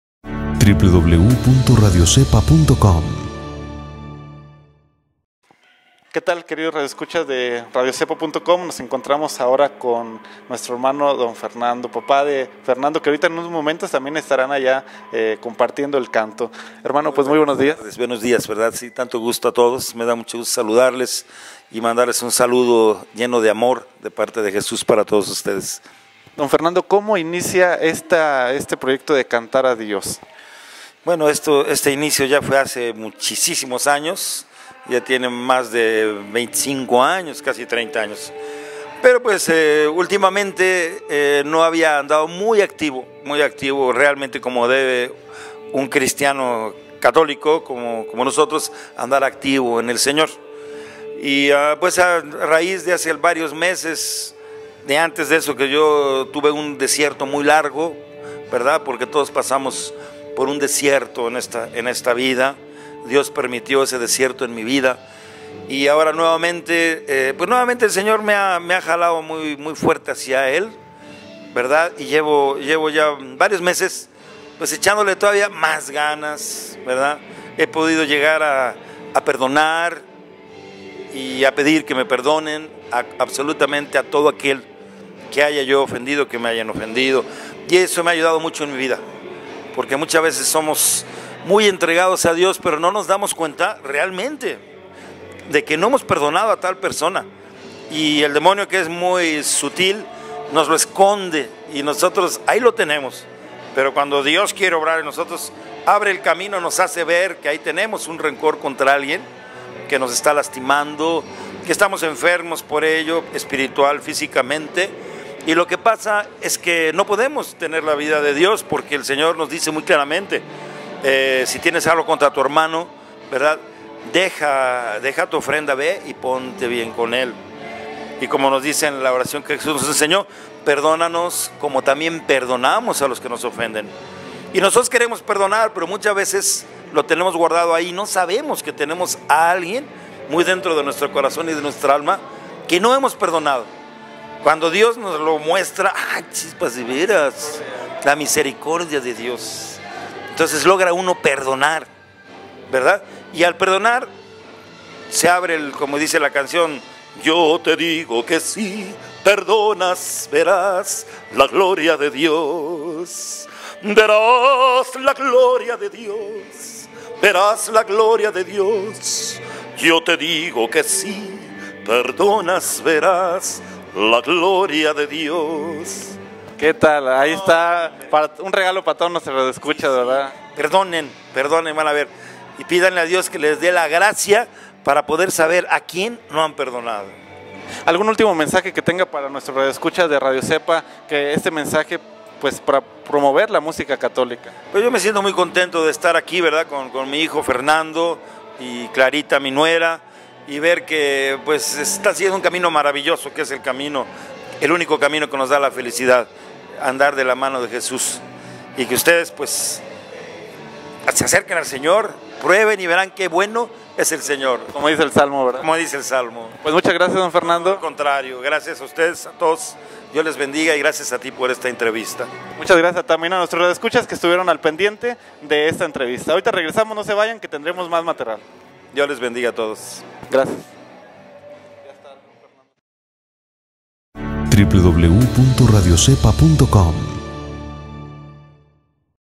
Fernando Kurezyn (entrevista)
Fernando Kurezyn es el papá de Fernando Kurezyn jr. Cantantes católicos radicados en Puebla México.

La música católica se comparte de generación en generación y se trasmite el mensaje  que Dios nos da.

Escucha su música y la de muchos cantantes en nuestra radio